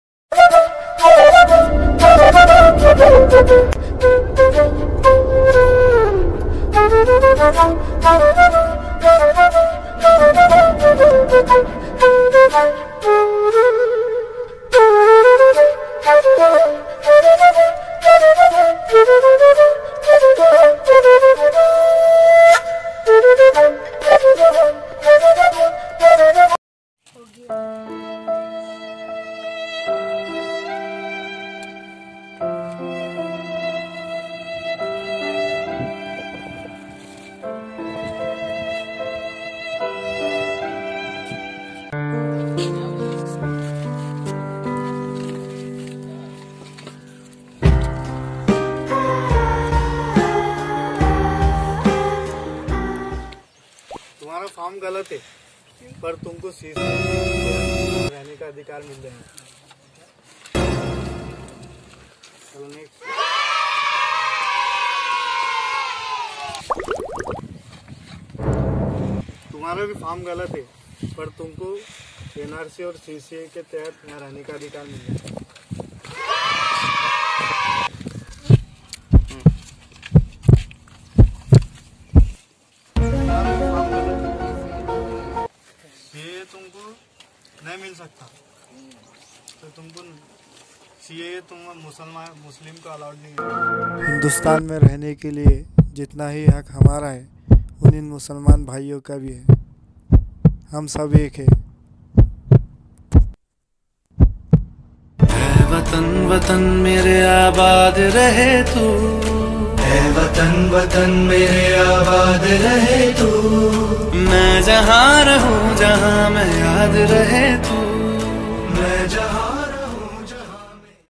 HAM SAB EK HAI ||HINDU ||MUSLIM |SIKH||ISAI|NO_NRC NO_CAA NO_NPR AAPAS ME HAM BHAI BHAI
Hame ummeed heke aapko hamari Ye ekta ki video pasand aayee hongi so guys video kaisee lagi ya video ka konsa scene Achchah laga please comment me batai love you all...